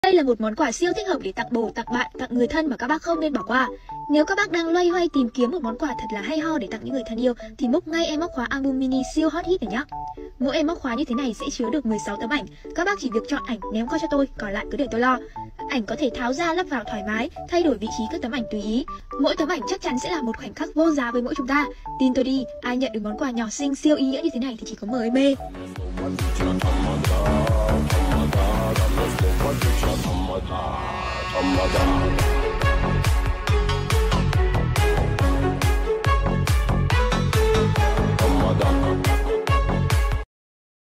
📸🔑 Móc khóa album ảnh mini
Một phụ kiện xinh xắn để thể hiện phong cách của mình.
Một cách tuyệt vời để mang theo những hình ảnh yêu thích của bạn mọi lúc, mọi nơi.
Với khả năng chứa đựng từ 5 đến 16 ảnh. Bạn có thể thay đổi ảnh theo sở thích hoặc tâm trạng.

✅✅ làm bộ tặng người yêu, treo khoá xe, treo cặp, balo đều được nè ❤️❤️
🤩MÓC KHÓA ALBUM MINI siêu xinhhh in hình theo yêu cầu~~~. ‼️‼️
👉Gói gọn cả thanh xuân vào một cuốn album móc khoá mini siêu đáng yêu😘❤️ Có 1 màu: trắng trong suốt kim tuyến lắp lánh chống nước 💯%

✔Móc khóa Album có 2 kích thước ạ:
❌ Kích thước 35x45mm (Tặng kèm ngay 16 ảnh) GIÁ 100k/ móc
❌ Kích thước 55x62mm (Tặng kèm ngay 18 ảnh) GIÁ 120k/ móc
Các bạn đặt móc khóa Album kích thước nào ib gửi ảnh cho mình nhé
Luôn tuyển ctv ib mình tư vấn nha